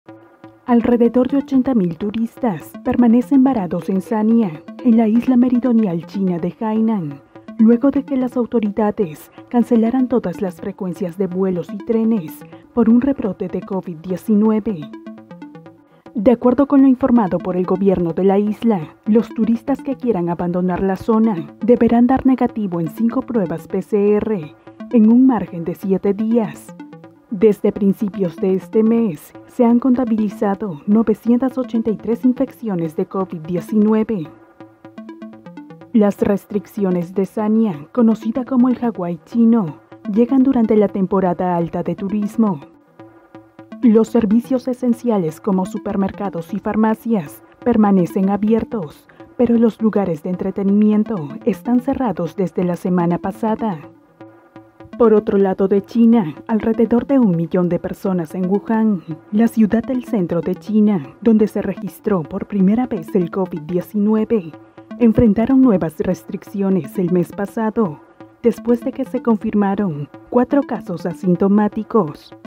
PBO INTERNACIONAL🌍| Sanya sigue confinado por un rebrote de COVID que dejó 80 mil turistas varados
INTERNACIONAL 🌍 PBO | China Sanya sigue confinado por un rebrote de COVID que dejó 80 mil turistas varados